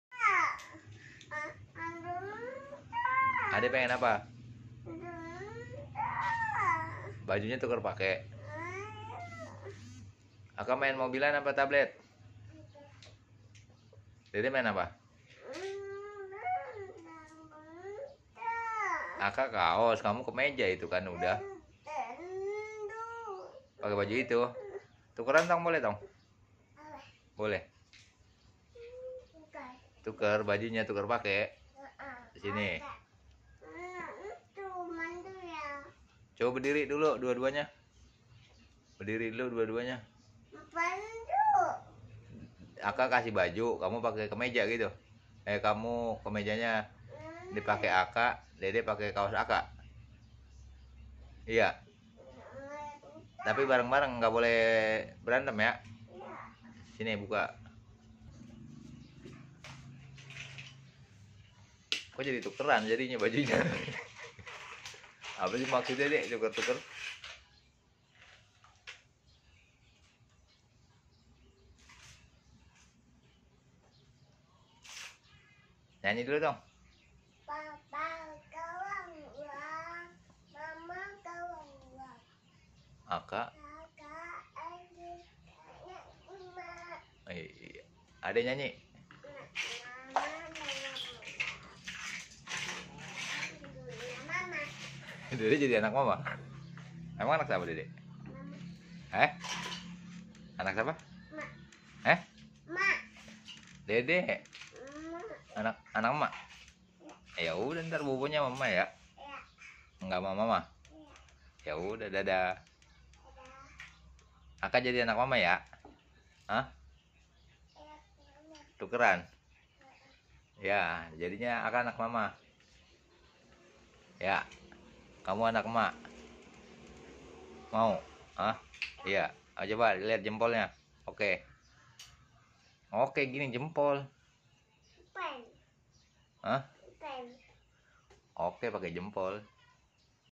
ANAK ANAK BERMAIN
Video ini hanya hiburan ketika saya mengasuh anak anak dan sambil mengajarkan mereka untuk belajar kompak...

Sukses untuk kita semua

Mohon bantuan nya untuk

____ SUBSCRIBE_____

Agar saya lebih kreatif mengembangkan channel ini